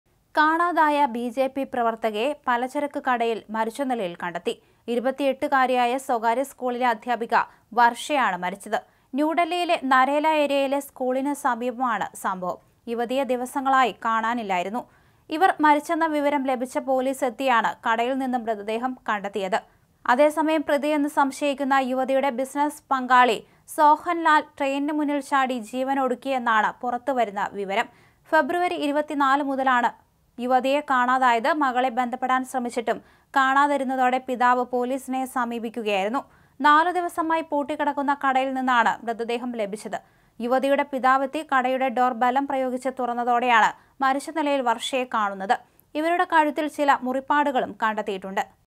ബിജെപി പ്രവര്‍ത്തക മരിച്ച നിലയില്‍ l Delhi s Narela area
കാണാതായ ബി.ജെ.പി പ്രവർത്തകയെ പലചരക്ക് കടയിൽ മരിച്ച നിലയിൽ കണ്ടെത്തി. 2സ്വകാര്യ സ്കൂളിലെ അദ്ധ്യാപിക വർഷ പവാർ (28) ആണ് മരിച്ചത്. ന്യൂഡൽഹിയിലെ നരേല ഏരിയയിലെ സ്കൂളിന് സമീപമണ് സംഭവം.